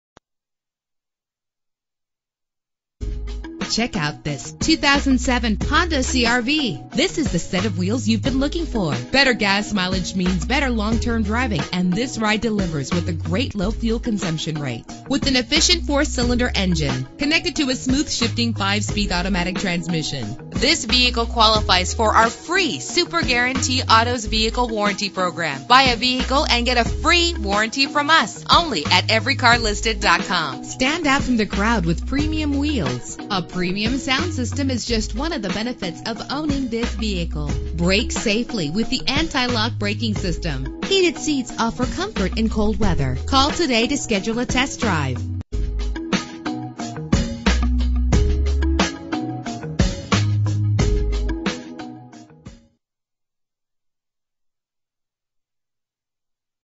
2007 Honda CR-V - Golden CO
Year: 2007
Make: Honda
Model: CR-V
Trim: EX-L
Engine: 2.4 liter inline 4 cylinder 16 valve
Transmission: 5-Speed Automatic
Color: Blue
Mileage: 59152
Address:
15601 W. Colfax Ave.